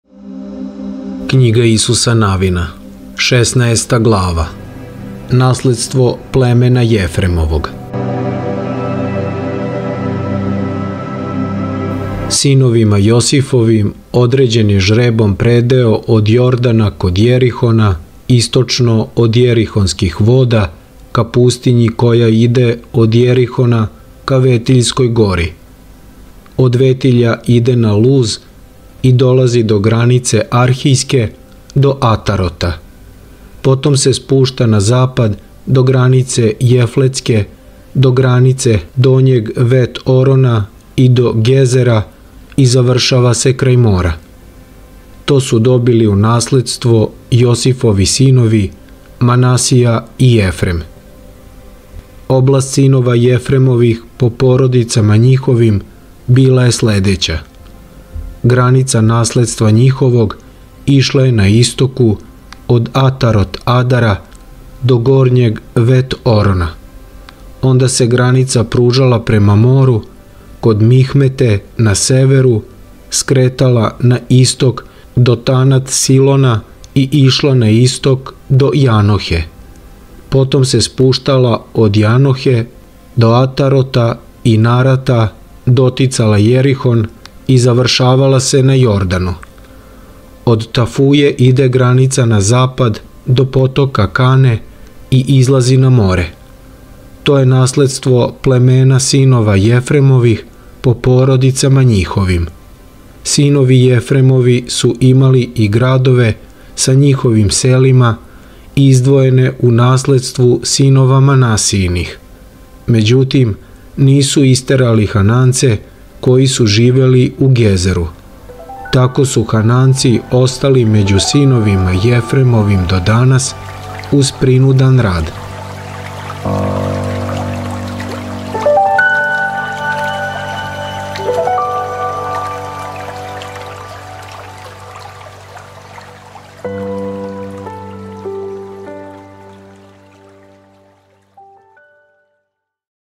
Књига Исуса, сина Навиновог, 16. глава / Наследство племена Јефремовог
Књига Исуса, сина Навиновог, 16. глава:
00:12    Наследство Јефремових синова